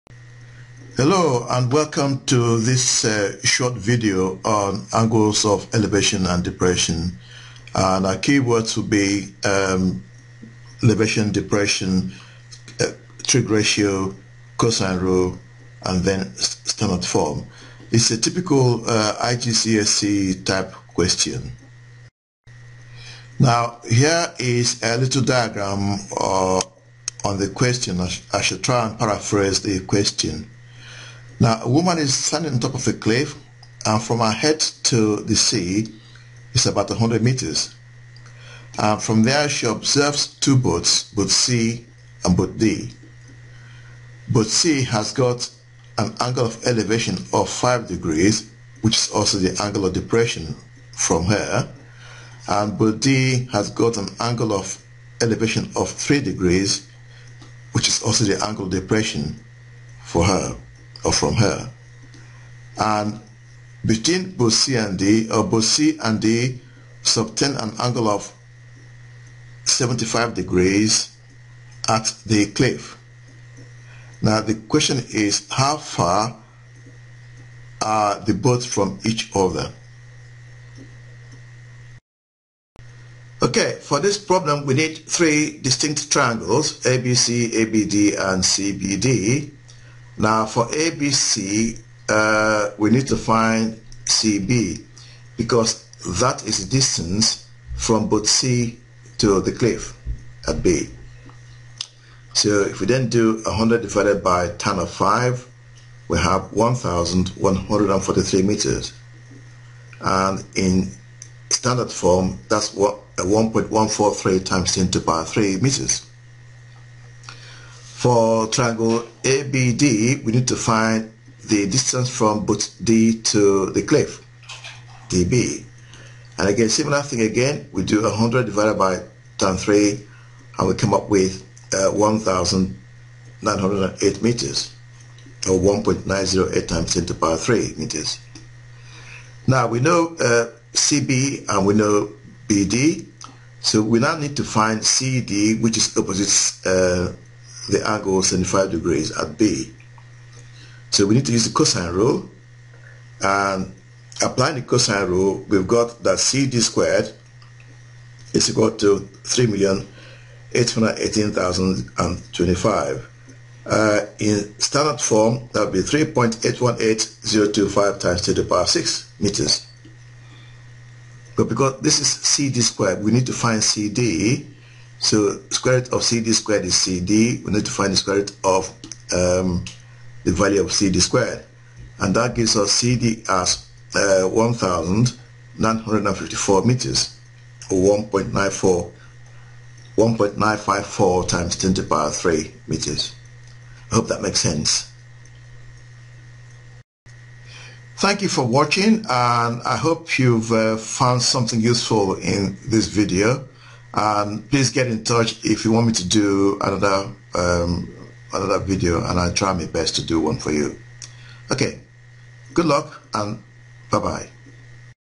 3D Maths:Angles of Depression and Elevation using trig ratio and cosine rule, IGCSE
In this video I have looked at a typical 3D question involving angles of depression and elevation.

Is there a topic or maths question you'd like me to help solve?